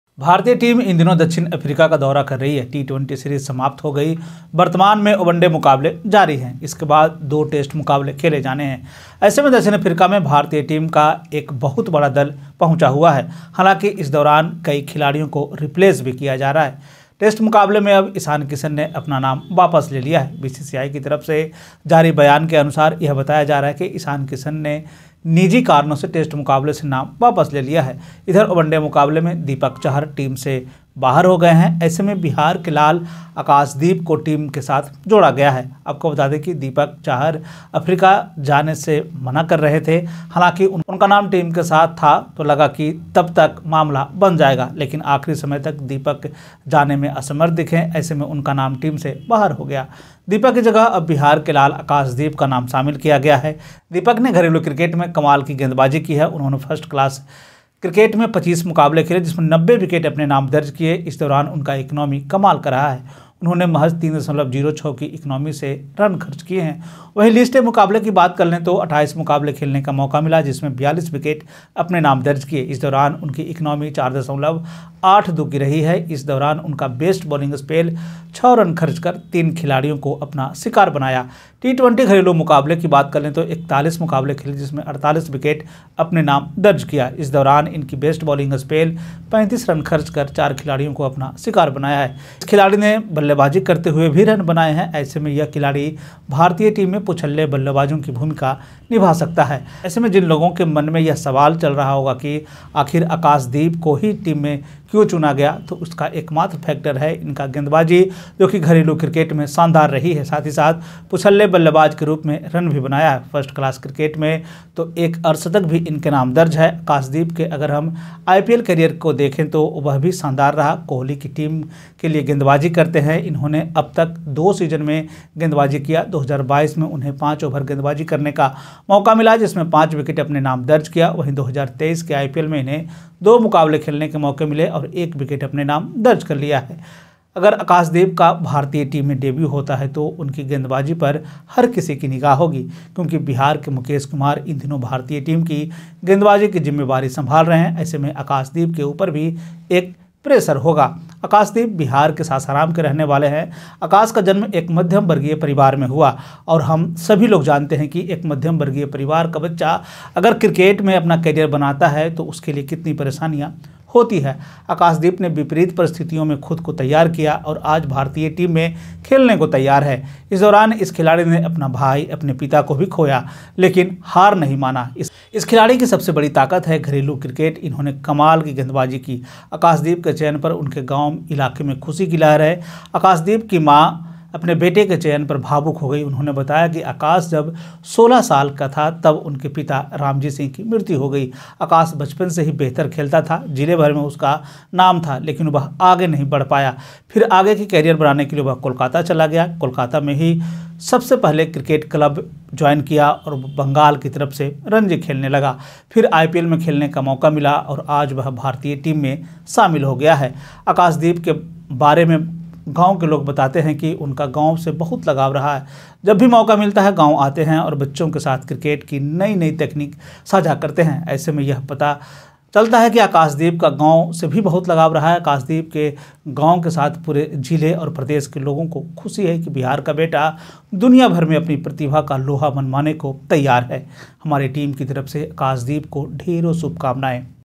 अर्शदीप के साथ अफ्रीकी बल्लेबाजों का शिकार करेगा अपना आकाशदीप
The name of Lal Akashdeep of Bihar has been included in place of Deepak. Deepak has bowled amazingly in domestic cricket. He has played 25 matches in first class cricket and has taken 90 wickets in his name. During this period his economy has been amazing, he has spent runs at an economy of just 3.06. Talking about List A matches, he got a chance to play 28 matches in which he took 42 wickets, during which his economy was 4.82. During this period, he has had a west bowling spell, costing 6 runs and making 3 players his victims. Talking about T-20 domestic matches, he has played 41 matches in which he has taken 48 wickets in his name. During this, his best bowling spell has been that he has made 4 players his victims by spending 35 runs. This player has also scored runs while batting, hence this player can play the role of tail batting in the Indian team. In such a situation, the question that might be running in the minds of those people is that why Akashdeep was selected in the team, then the only factor is his bowling which has been excellent in domestic cricket. He has also scored runs as a tail batsman. He also has a half-century to his name in first class cricket. If we look at Akashdeep's IPL career, it has also been excellent.